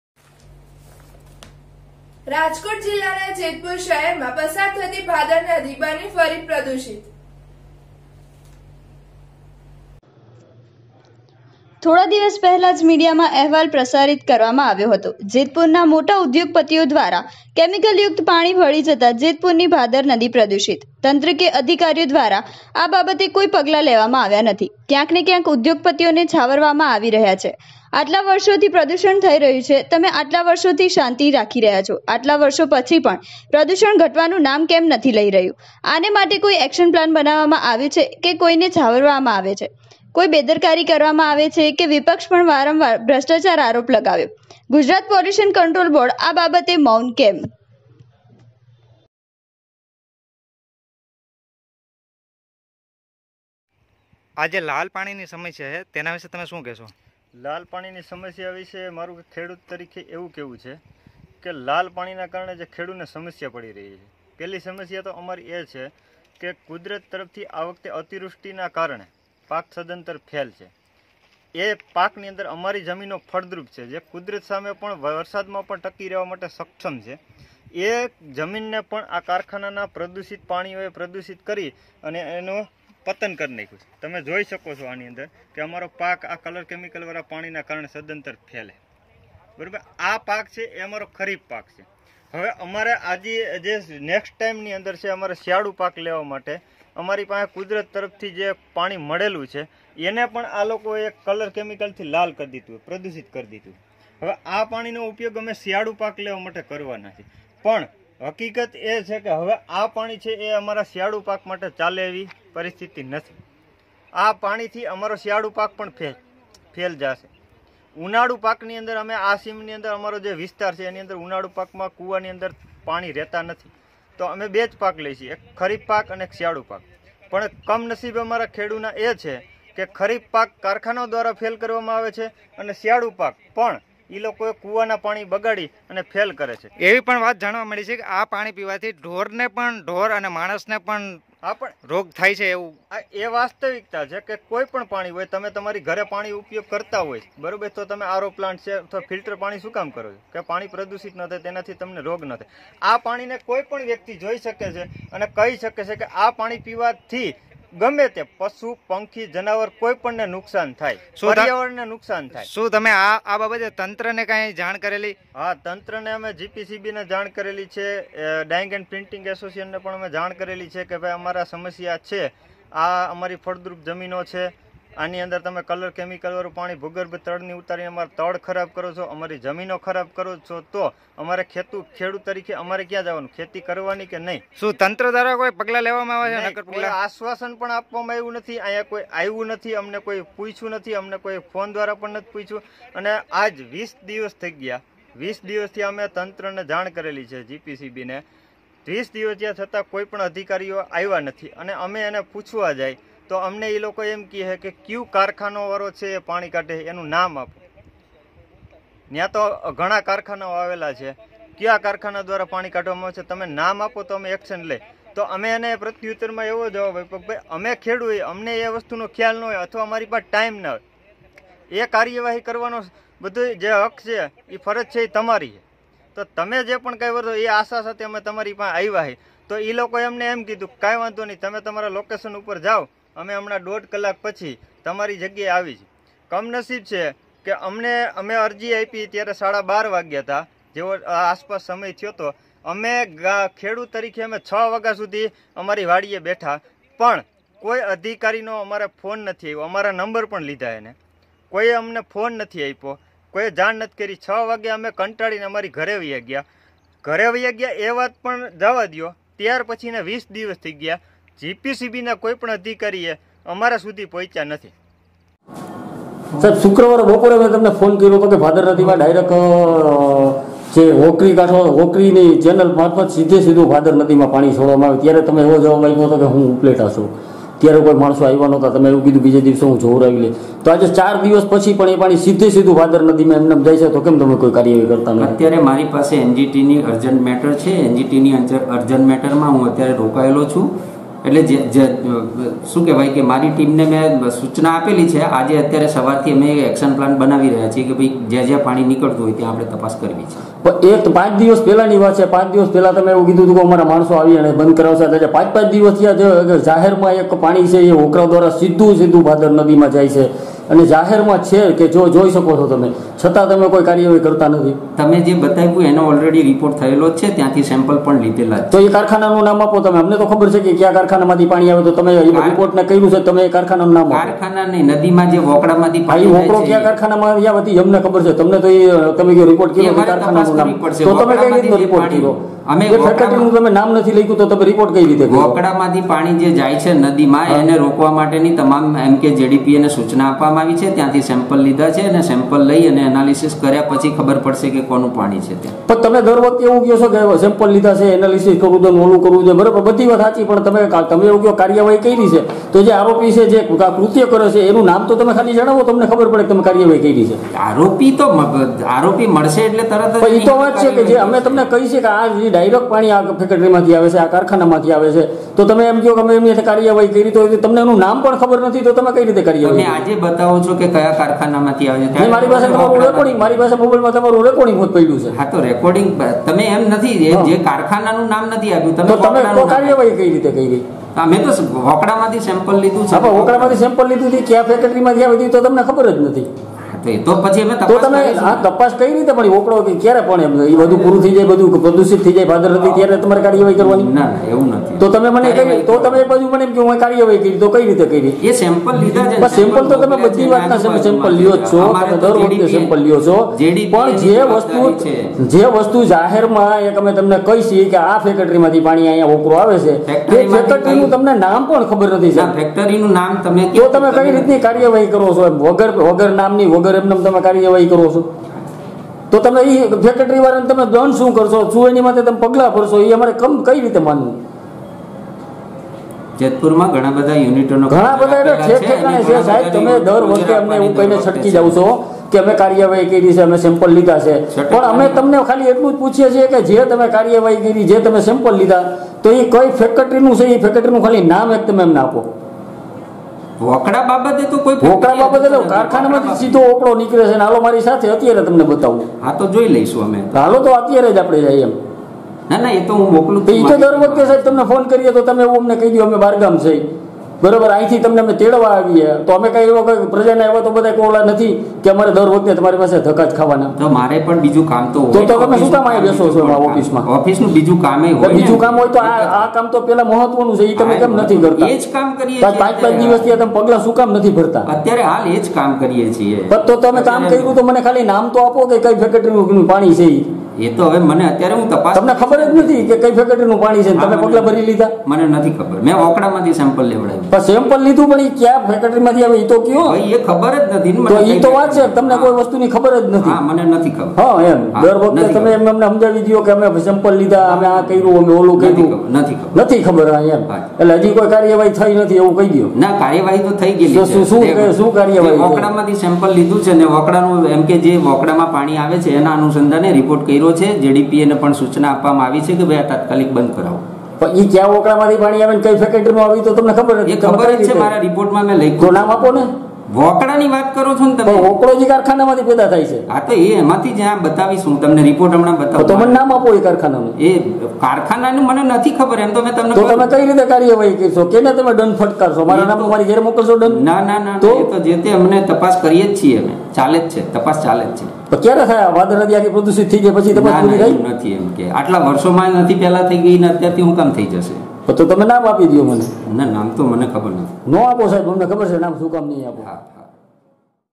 SPEEDNEWS17 રાજકોટ જીલ્લ્લાના જેતપુર શહેરમાં પસાર થતી ભાદર નદી બની ફરી પ્રદુષિત
રાજકોટ જીલ્લ્લાના  જેતપુર શહેરમાં પસાર થતી ભાદર નદી બની ફરી પ્રદુષિત

થોડા દિવસ પહેલા જ મીડિયામાં અહેવાલ પ્રસારિત કરવામાં આવ્યો હતો

જેતપુરના મોટા ઉધોગપતિઓ  દ્વારા કેમિકલયુક્ત પાણી ભળી જતા જેતપુરની ભાદર નદી પ્રદુષિત

તંત્ર કે અધિકારીઓ દ્વારા આ બાબતે કોઈ પગલાં લેવામાં આવ્યા નથી

ક્યાંક ને ક્યાંક ઉધોગપતિઓને છાવરવવામાં આવી રહ્યો છે

આટલા વર્ષોથી પ્રદૂષણ થઈ રહ્યું છે તમે આટલા વર્ષોથી શાંતિ

 આટલા વર્ષો પછી પણ પ્રદૂષણ ઘટવાનું નામ કેમ નથી લઈ રહ્યું

 આના માટે કોઈ એક્શન પ્લાન બનાવવામાં આવ્યું છે કે કોઈ ને છાંવરવામાં આવે છે

કોઈ બેદરકારી કરવામાં આવે છે કે

વિપક્ષ પણ વારંવાર ભ્રષ્ટાચાર આરોપ લગાવ્યો

ગુજરાત પોલ્યુશન કન્ટ્રોલ બોર્ડ આ બાબતે મૌન કેમ?..,